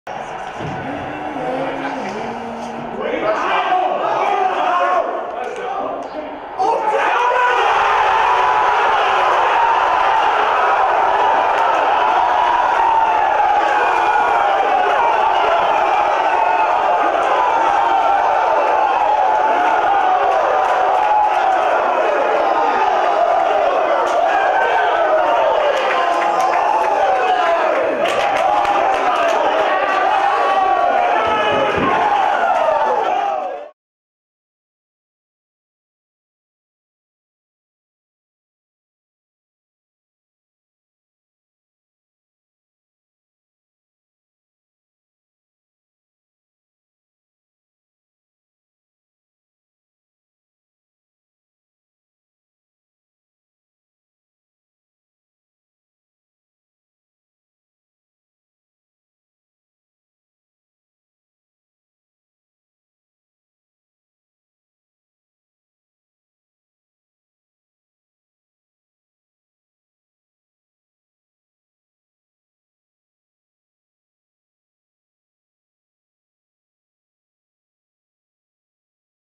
Wisconsin Football reacts to Cobb Touchdown
The Badgers gathered to watch the Packers-Bears game at the team hotel in Orlando. Here's the reaction to Randall Cobb's touchdown catch on fourth down, sealing the playoff berth for the Packers.